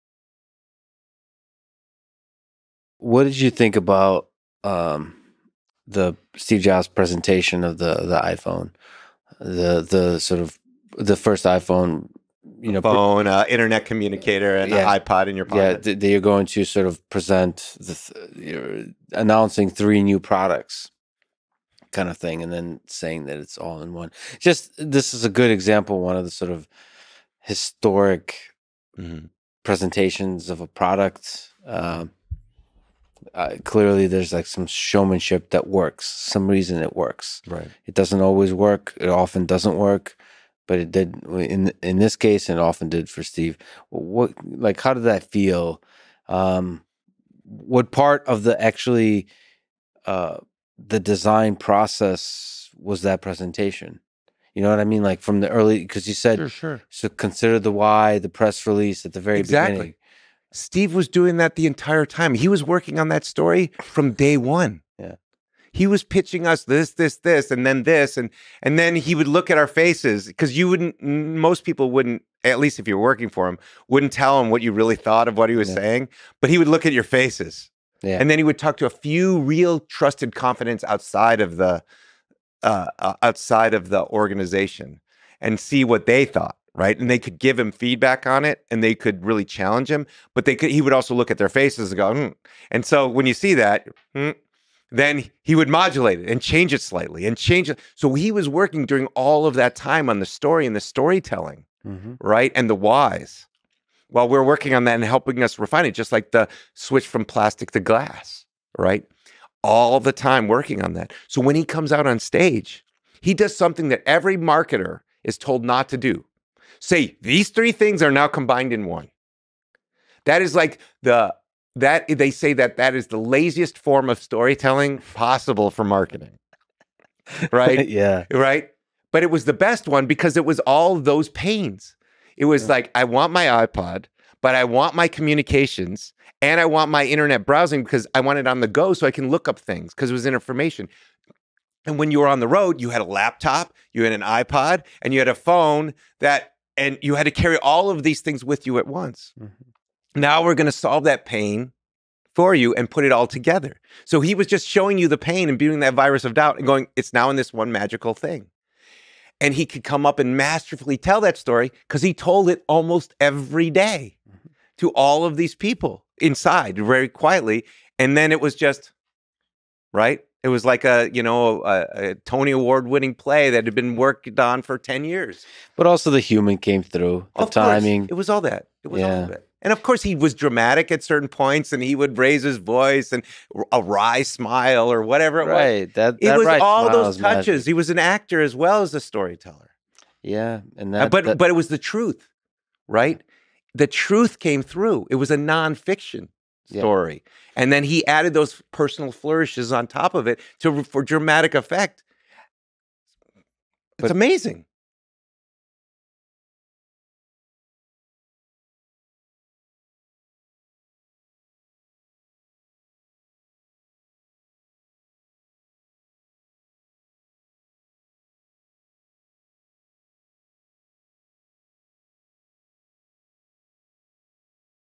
Steve Jobs reveals the first iPhone | Tony Fadell and Lex Fridman
- NetSuite to get free product tour
- SimpliSafe and use code LEX

GUEST BIO:
Tony Fadell is an engineer and designer, co-creator of the iPod, iPhone, Nest Thermostat, and author of the new book Build: An Unorthodox Guide to Making Things Worth Making.